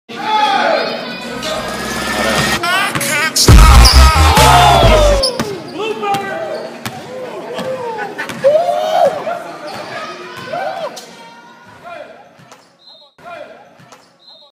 I can't stop basketball dunk fail
Javon brown going for a dunk on a wet floor.